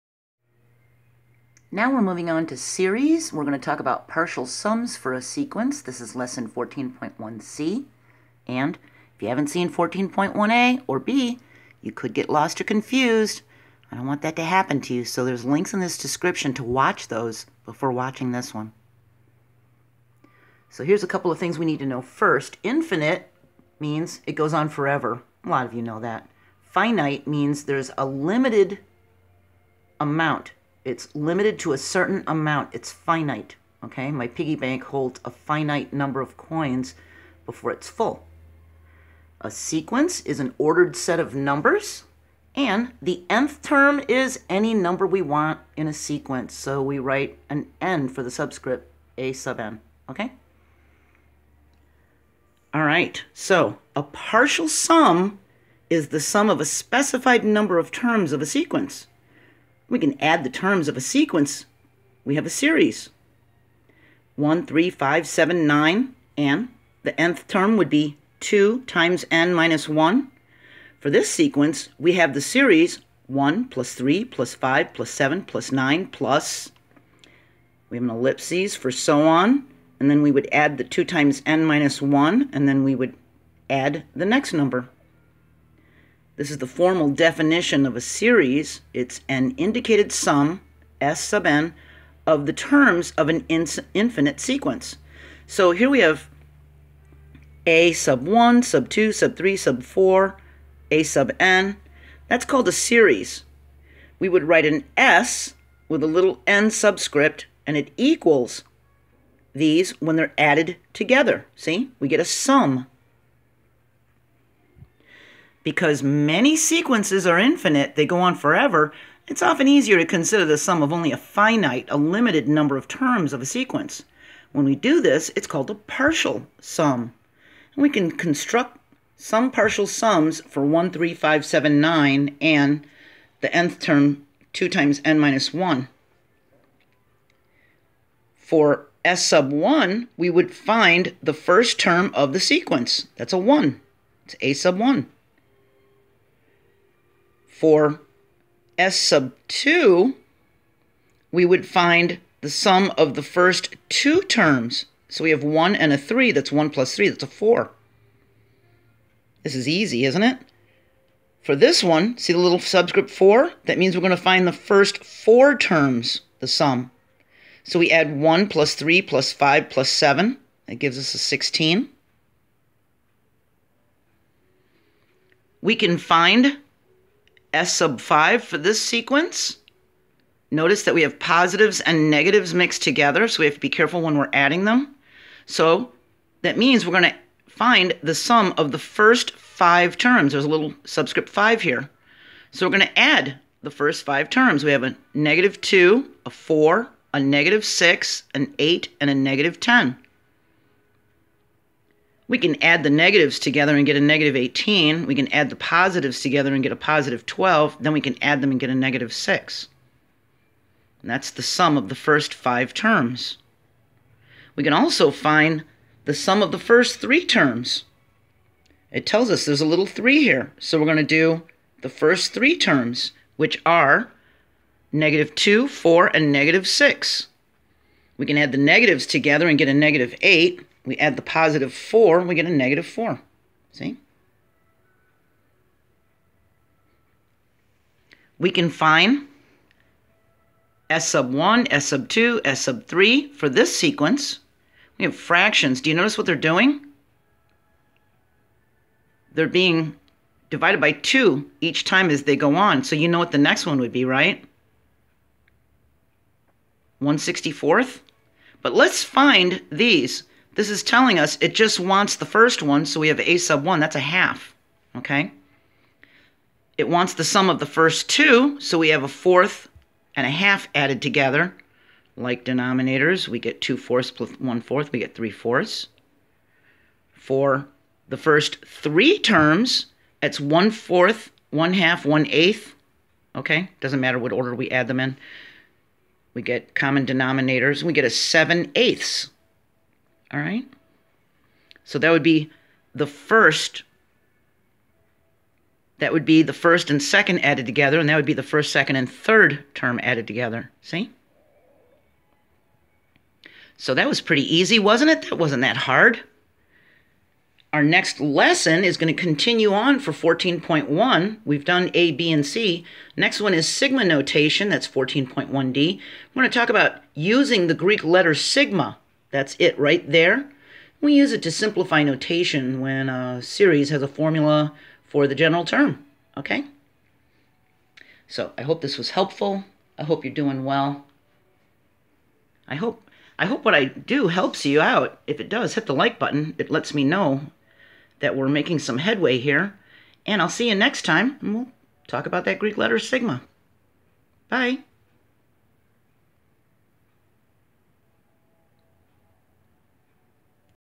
Algebra II 14.1c, Series (partial sums of sequences)
An explanation of Partial Sums, the definition of a Series, infinite series and finite series, finding the sum of the first few terms of a sequence, constructing partial sums for a sequence.

Playlist of the previous Lessons